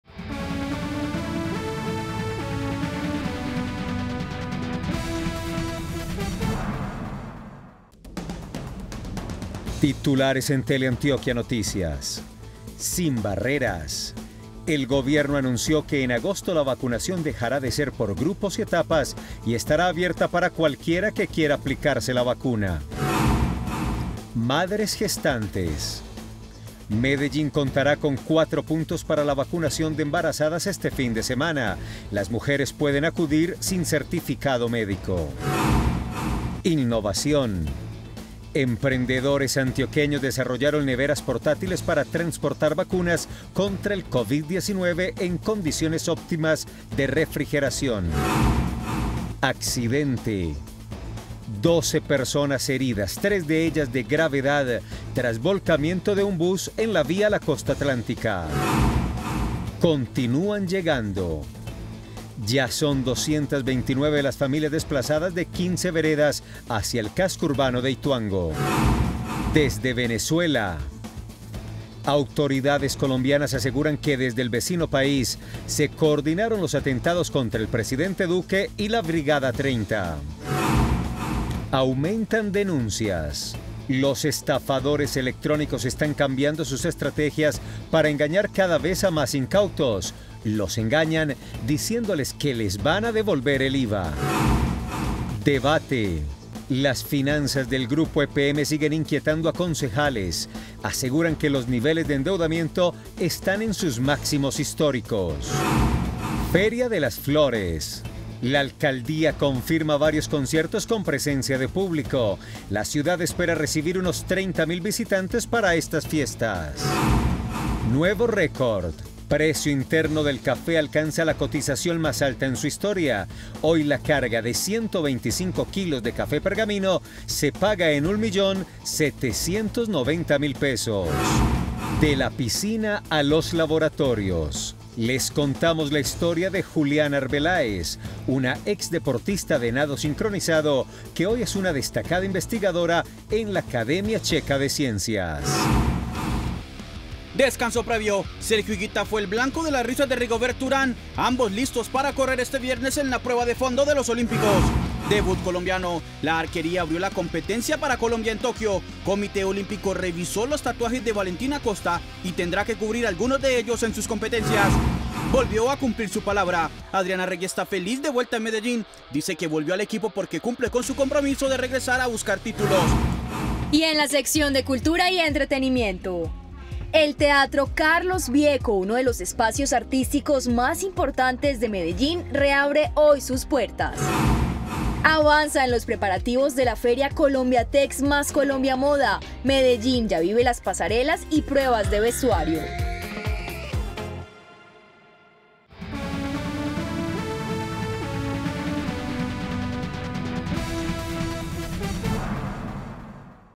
Titulares de Teleantioquia Noticias - jueves 22 de julio de 2021
Noticias. En agosto la vacunación sería para todas las edades. Cuatro puntos para vacunación de embarazadas. Desarrollaron nevera portátil para vacunas Covid-19. Hay reducción en la mortalidad por Covid en todas las edades. Doce personas lesionadas por volcamiento de bus. Desde 15 veredas han llegado desplazados a Ituango. Conclusiones sobre atentados contra brigada y presidente. Cuidado: aumentan denuncias por fraude electrónico. “Endeudamiento de EPM está al límite”: concejales.